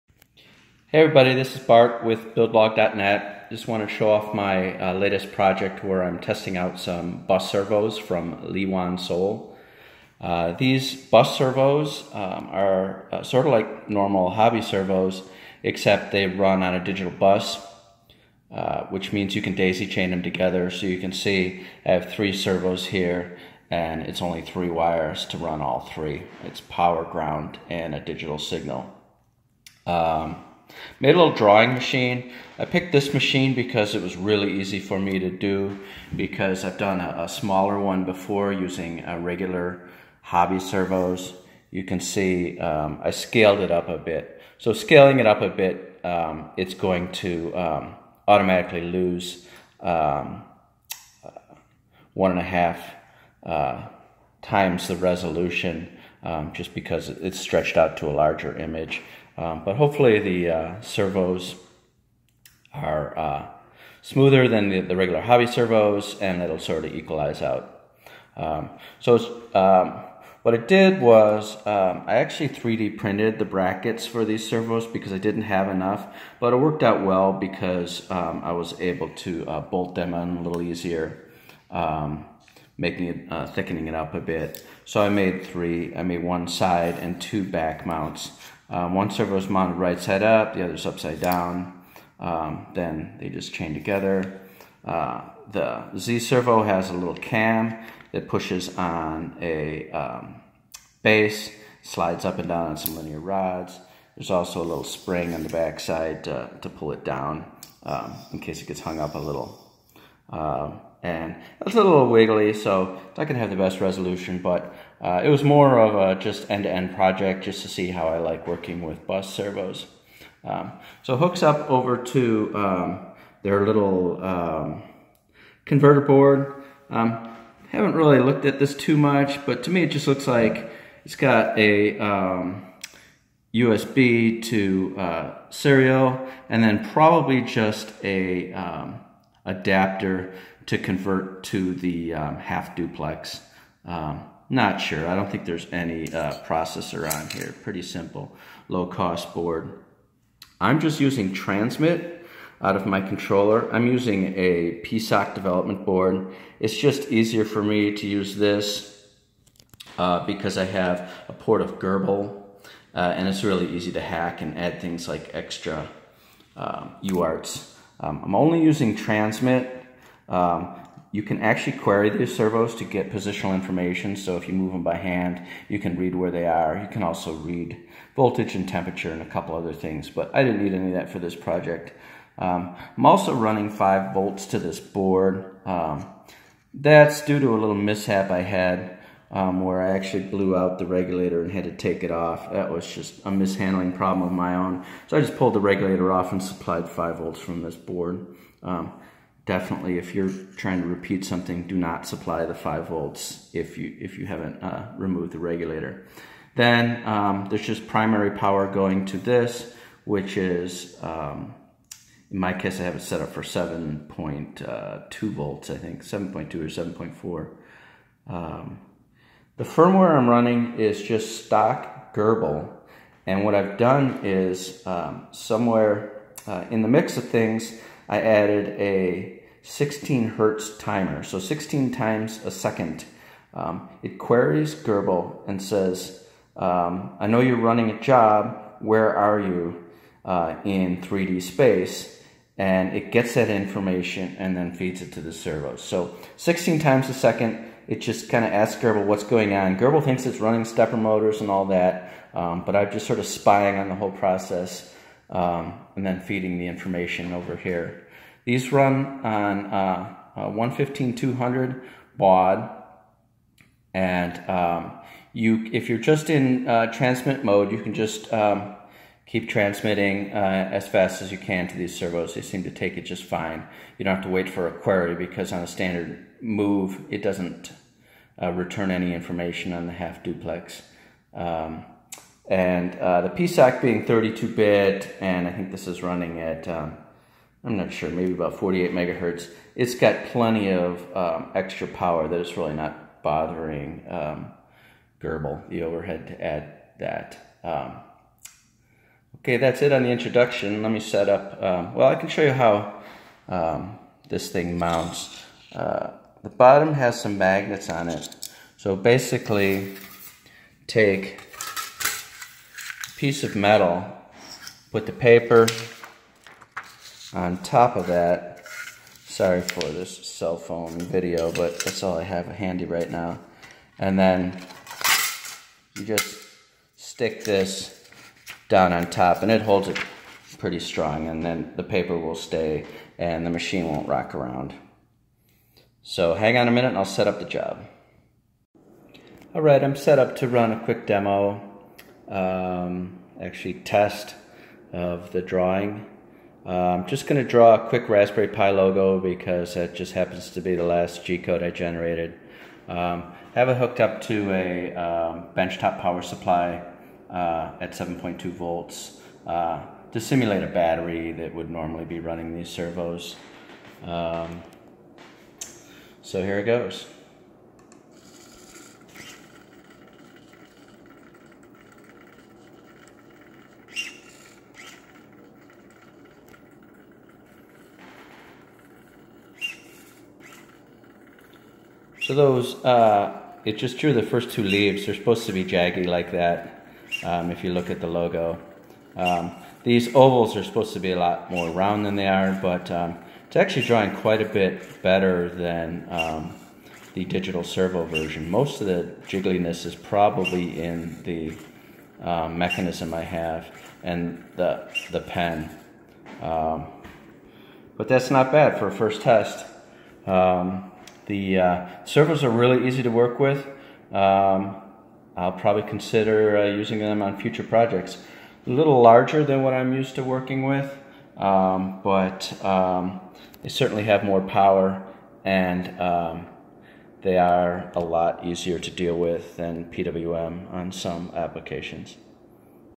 LewanSoul Bus LX-16A Servo Draw Bot
This is my first test of the Lewan Soul low cost bus servos.  See my web site for further details. @buildlog